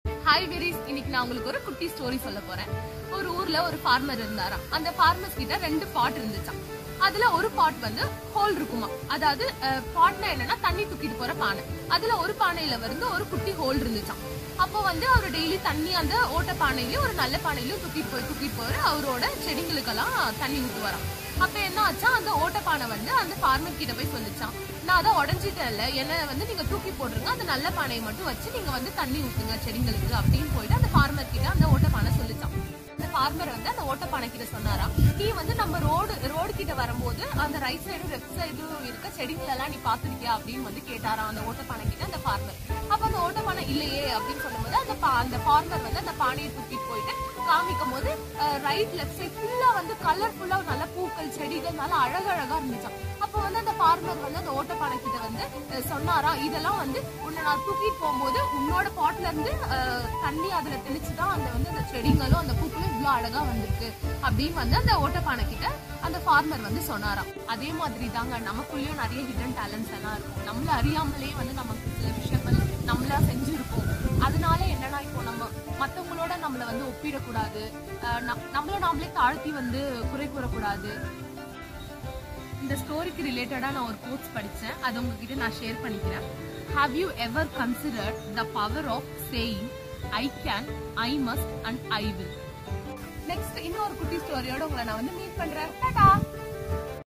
Kutty story - 1 "Know your worth"🙋‍♂️
You are who, you are🤗
Don't compare yourself with others 🤫
Don't develop hatred towards yourself🫠
Always be confident on yourself 🤩